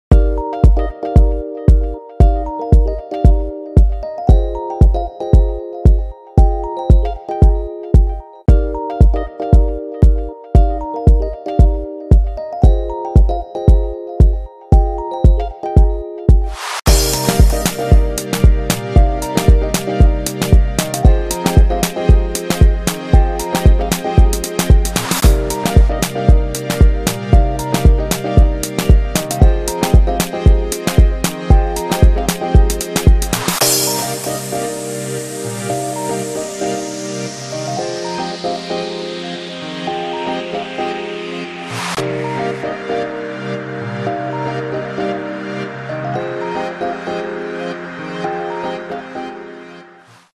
Dog Breed / Chow chow / Cream / Big Bones / Blue Tongue / Canine / Lipa city , Batangas Philippines
Meet Chow Chow Puppy, Cream Color , Big Bones, Short Snout , Lipa city , Batangas
Philippines